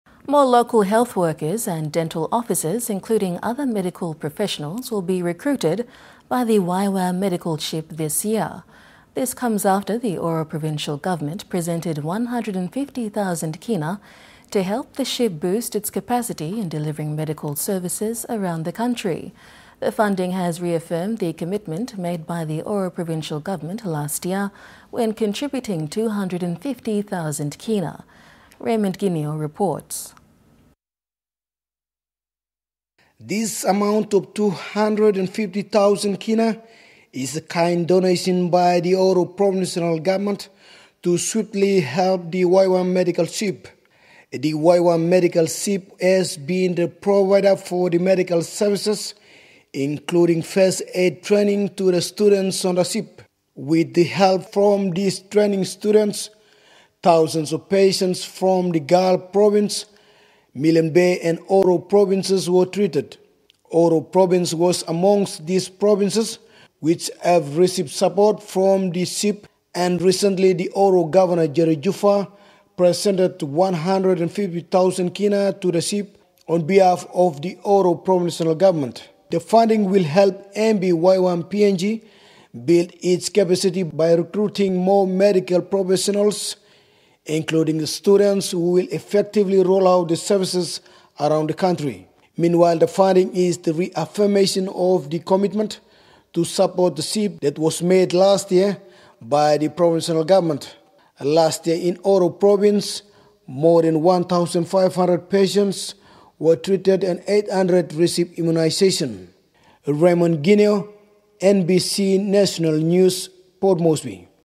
NBC News - YWAM recruits more
More local health workers and dental officers including other medical professionals will be recruited by the YWAM Medical Ship this year